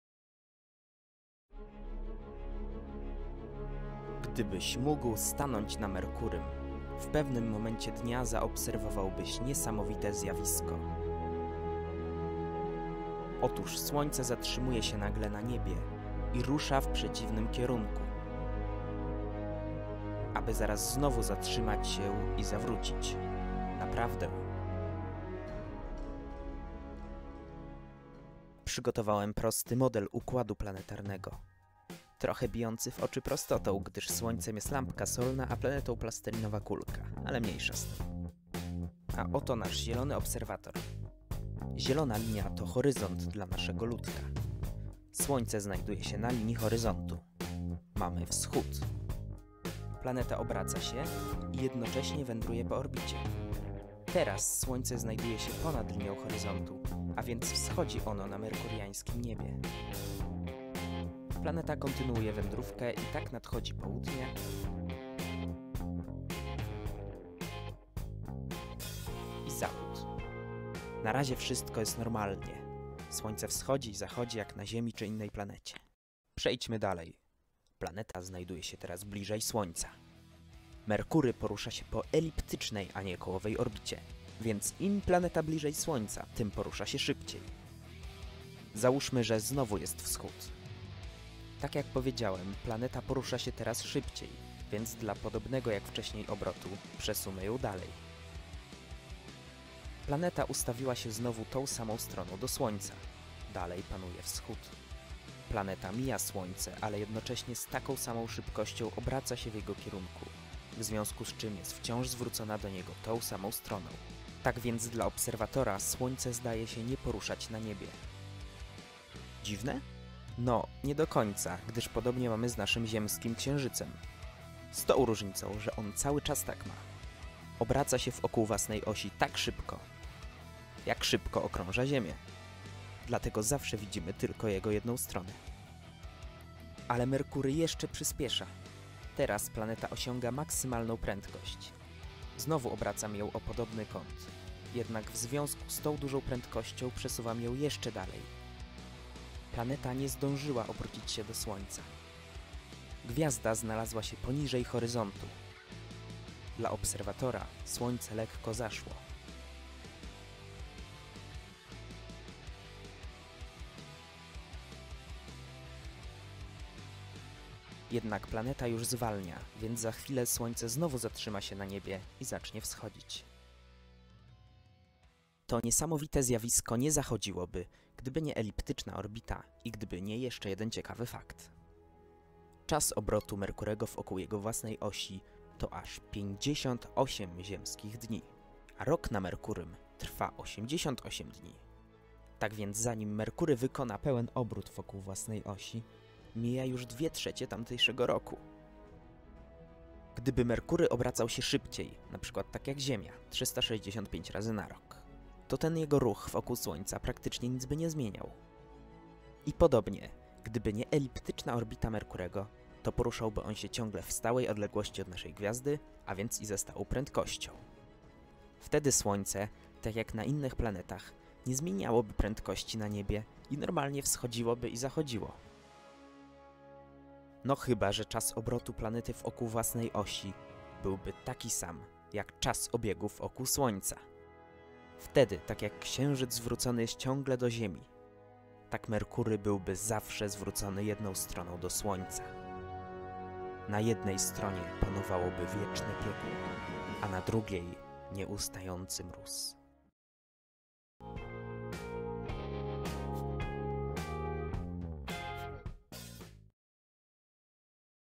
Zawracające Słońce na Merkurym
Niesamowite zjawisko cofania się Słońca na merkuriańskim niebie.